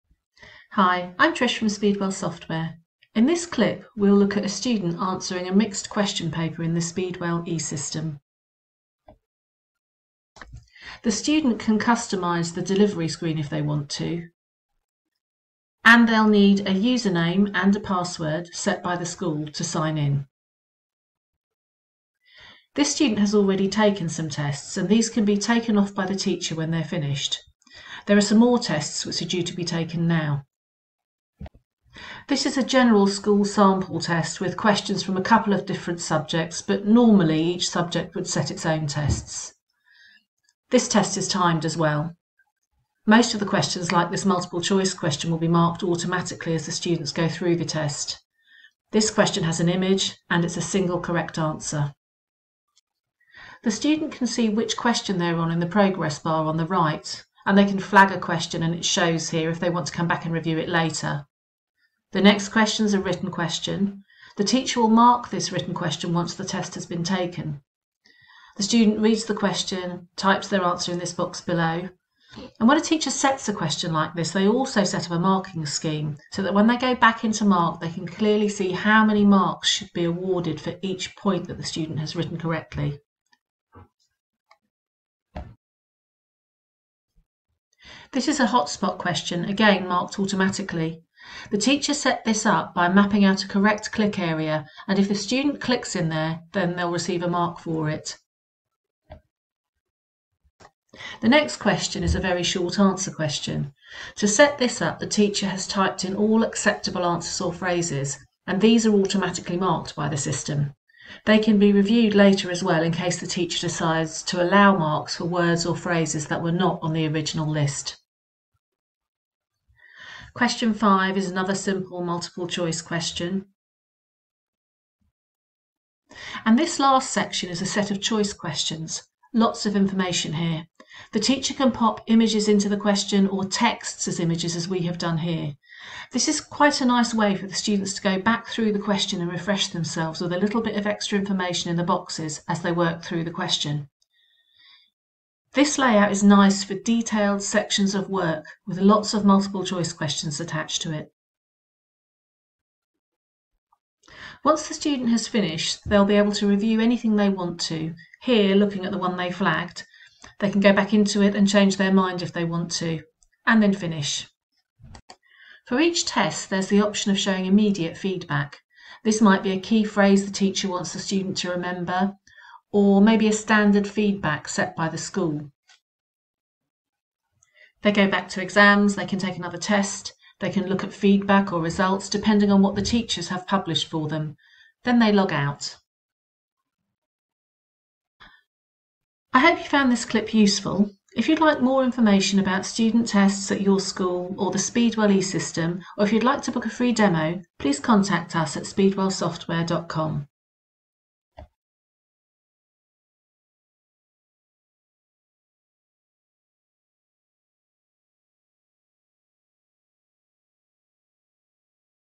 Exam Software for Schools - Student taking a test
See how eSystem exam software can be used for online exams in schools. This video shows a student taking an online test in eSystem.